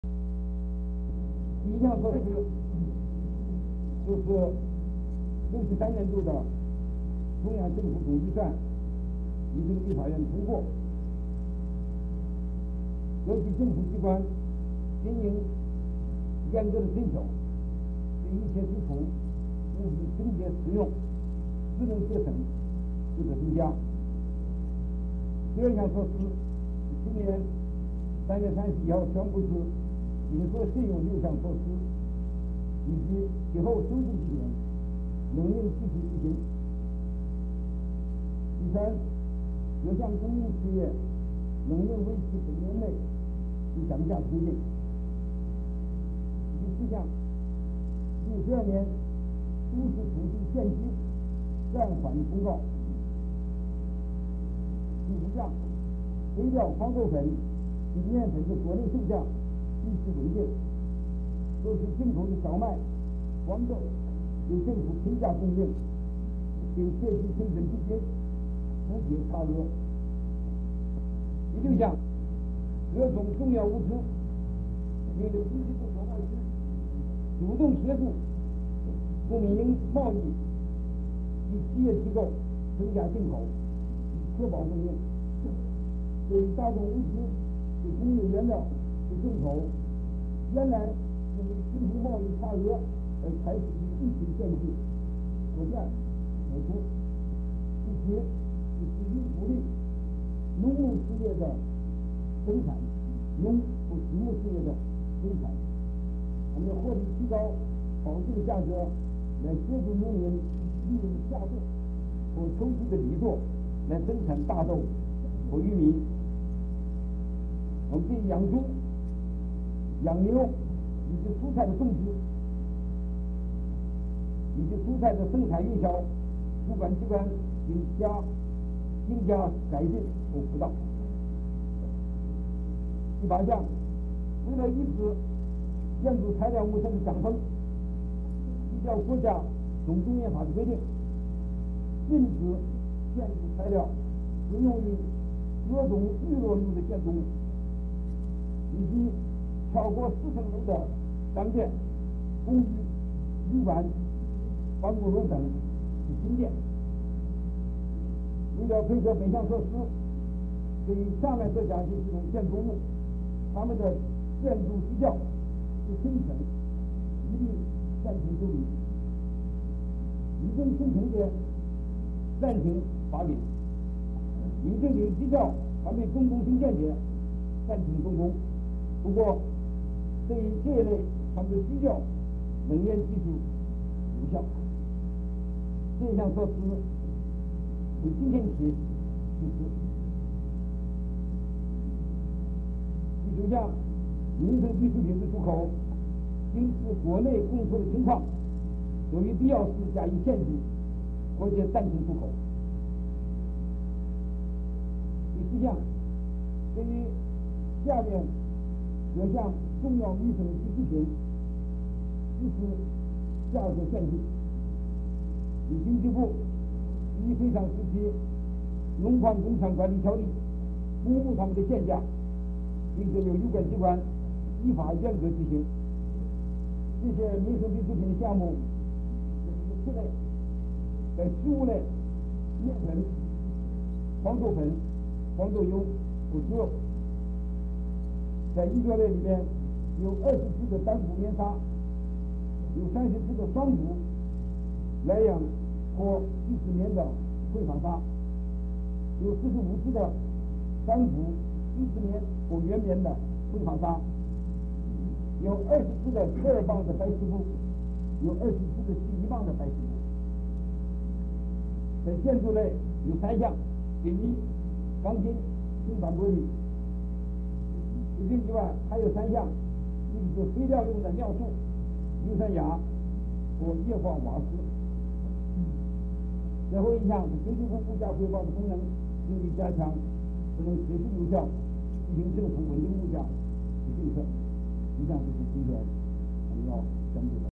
孫運璿部長談穩定物價
(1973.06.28)孫運璿部長談穩定物價十一項措施